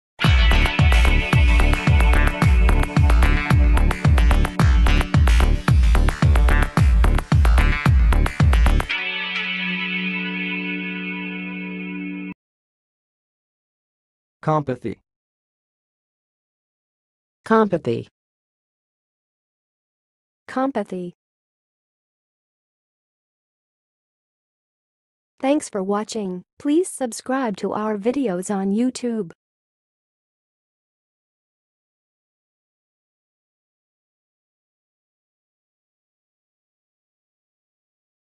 How to say "compathy"! (High Quality Voices)
Watch in this video how to say and pronounce "compathy"!